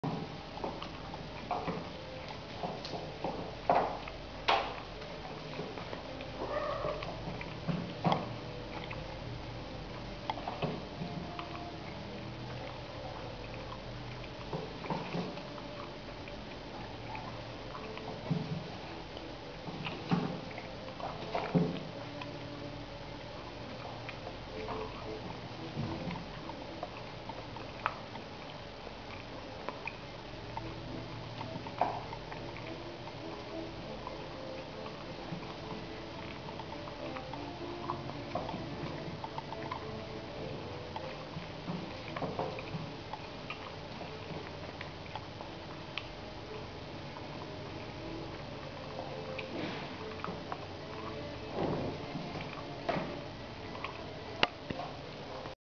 Chasing the Laser Pointer
Jay the Cat Sitter brought them a laser pointer as a present. They absolutely loved it.